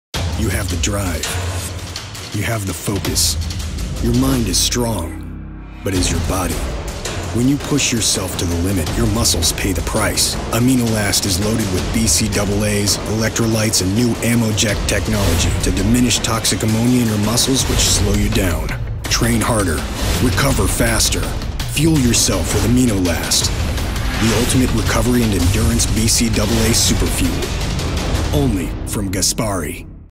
Best Company presenta lo spot ufficiale di Aminolast di Gaspari Nutrition
Aminolast di Gaspari Nutrition è un integratore a base di amminoacidi in grado di favorire la resistenza e di massimizzare la delicata fase di recupero muscolare. La formulazione di questo interessante integratore ha richiesto ben 24 mesi di sviluppo basato su recenti ricerche e tecnologie all'avanguardia.